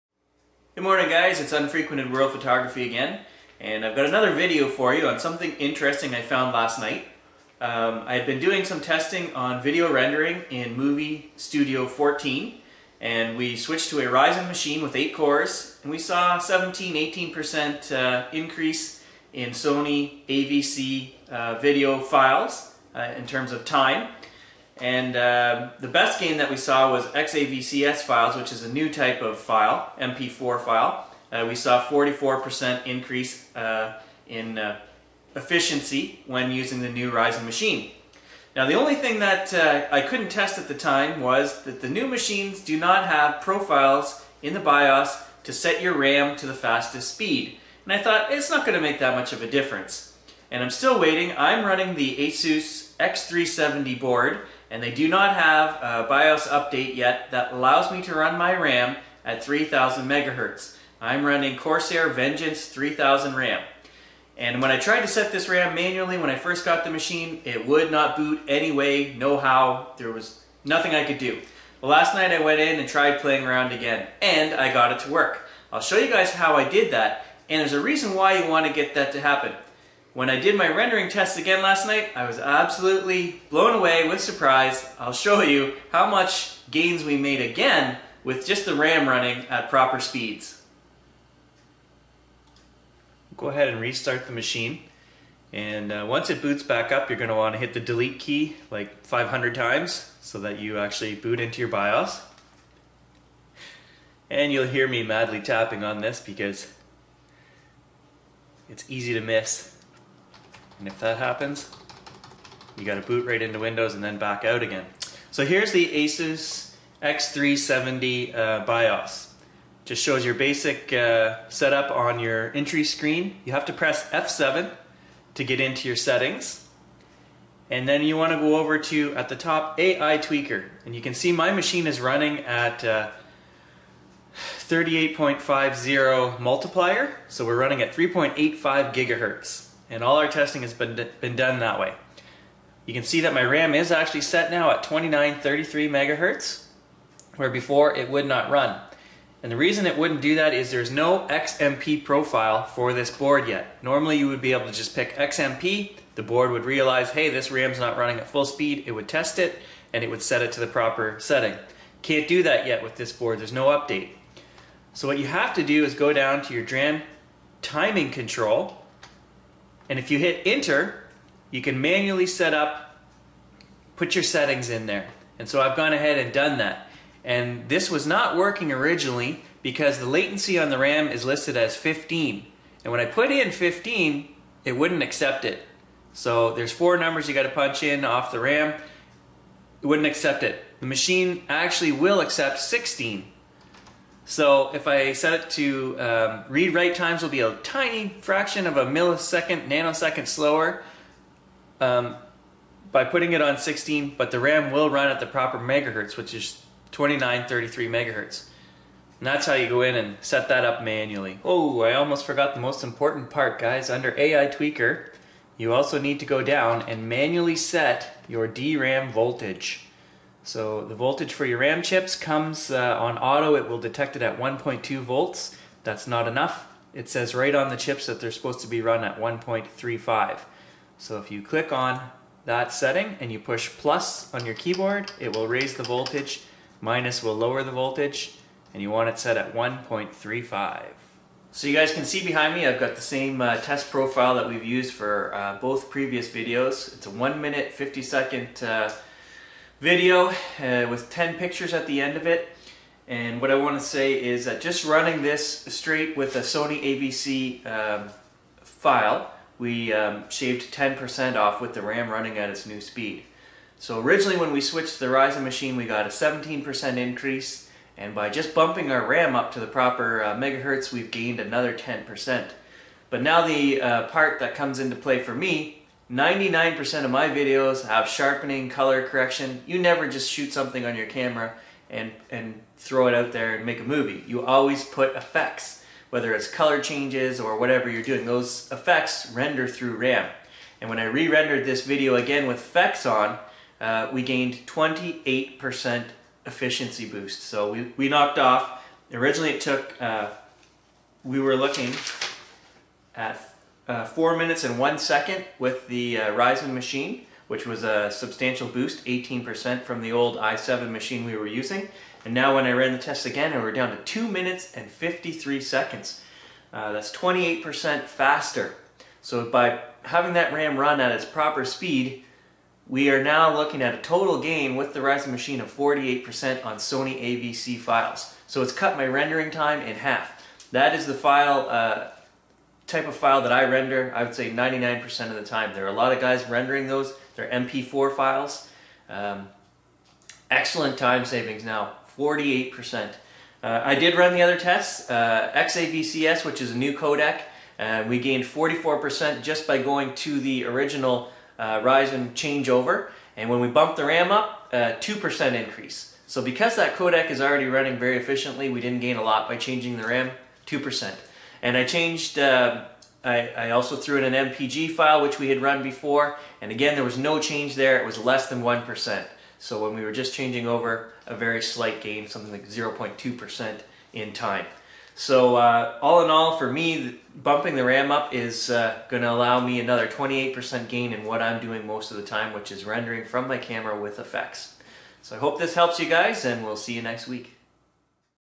Vegas Movie Studio 14 Ryzen Memory Test
How much performance gain will you see when rendering Sony AVC files on a new Ryzen 8 core machine with the memory profile set to its proper 3000hz. Well you might be surprised. I was. It is substantial. Join me and I'll show you how to set your ram profile manually and then we'll discuss how much time you'll save when rendering in Movie Studio 14.